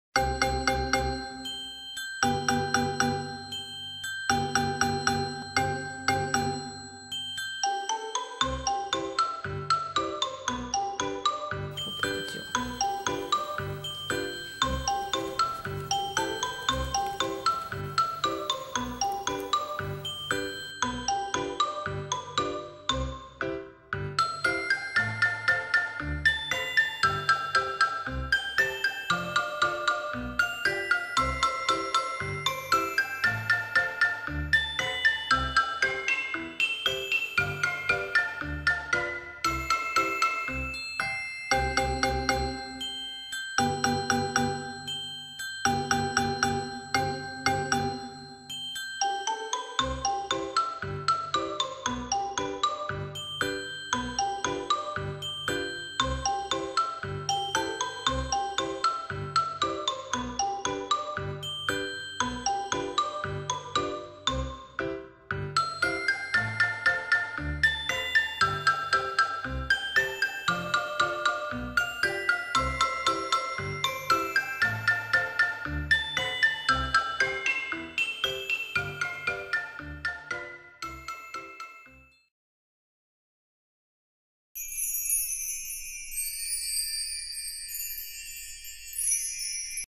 【ふなっしー石けん】作ってみた funassyi soap
作成期間1ヶ月・・・休みの度にちょこまか作って、ようやく形になりました。今思えばずいぶん無駄な作業も多かったのですが、それも手作りならではです。